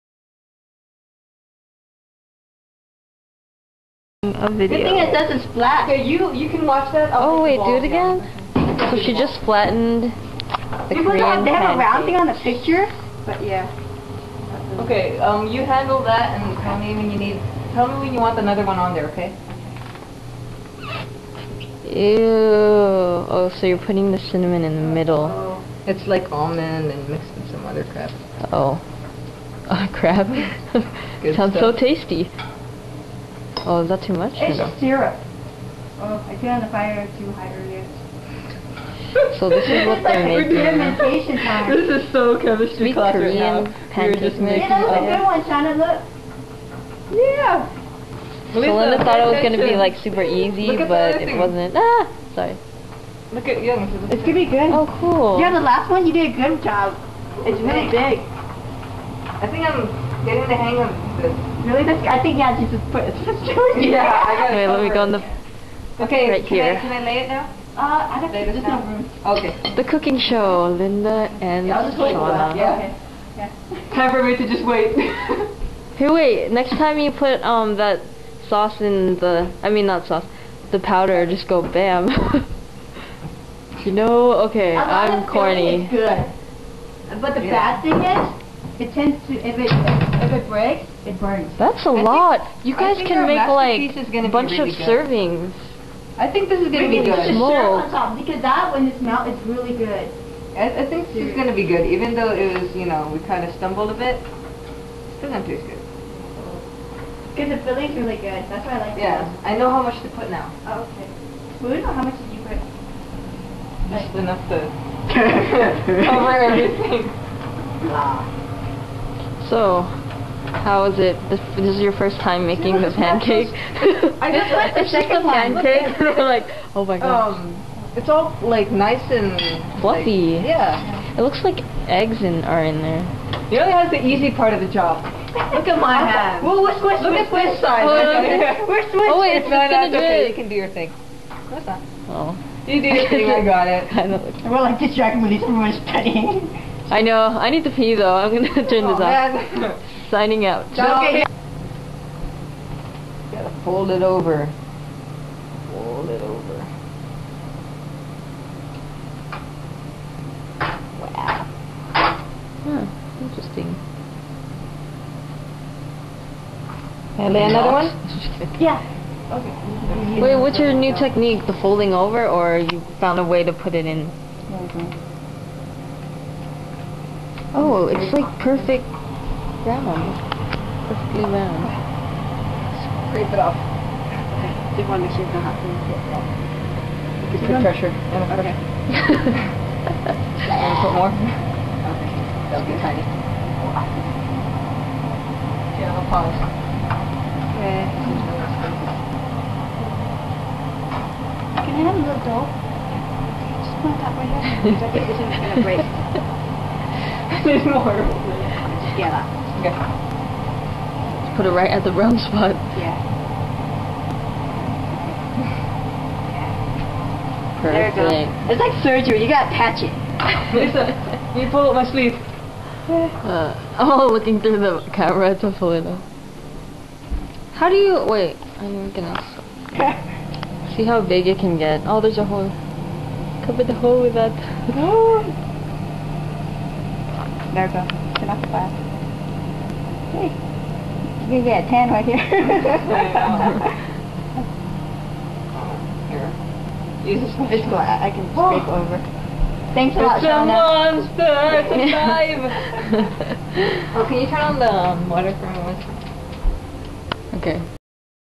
College Life: Cooking Hotteok
We didn't quite follow the directions correctly as they were in Korean. The dough is not supposed to be that sticky had we let it sit to allow the yeast to work. For some reason we didn't bother to check online. Nevertheless, it turned out delicious!